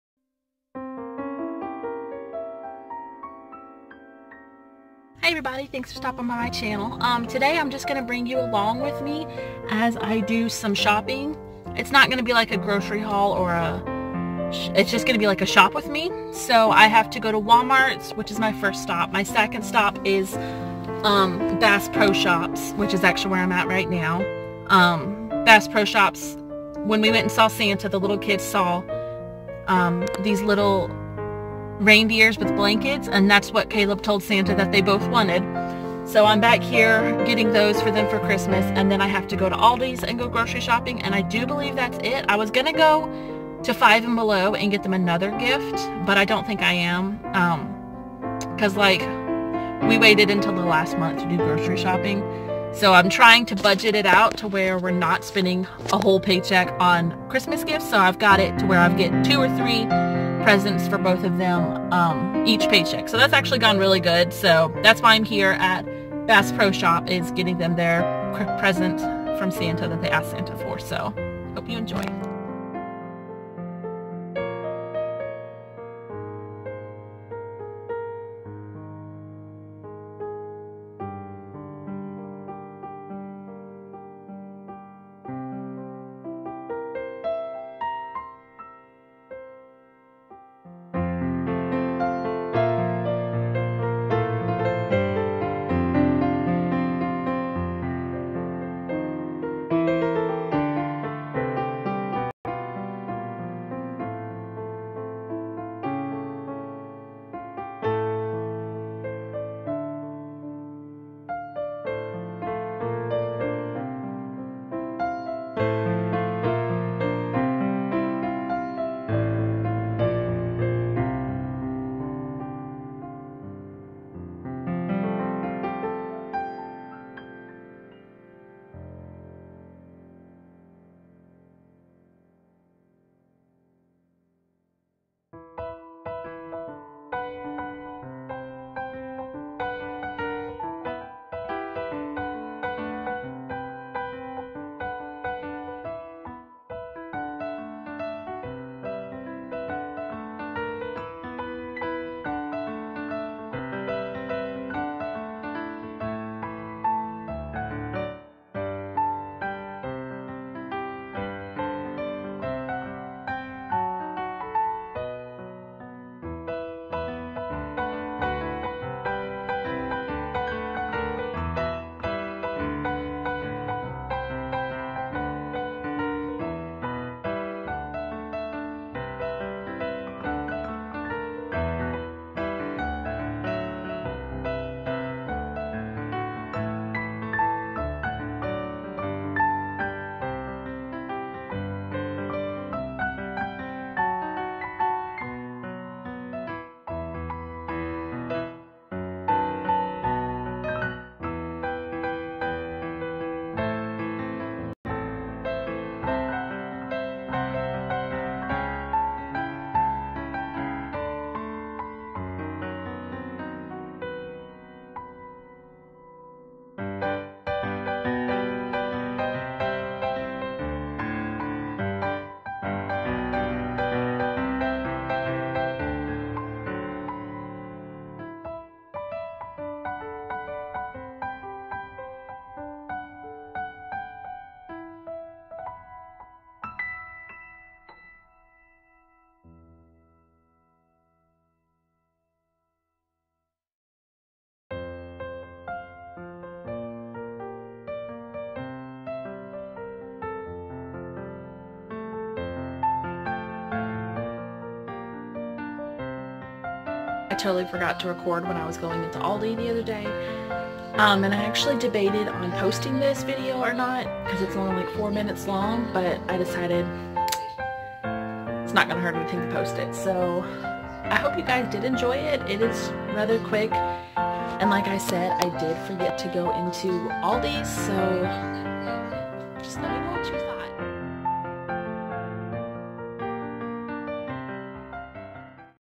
Shop With Me ☆ Small Shopping Trip
I know it's very short but I do hope you enjoy this show with me video!

I was planning on recording in Aldi but I forgot to record anything, it didn't help that the store was so crowded!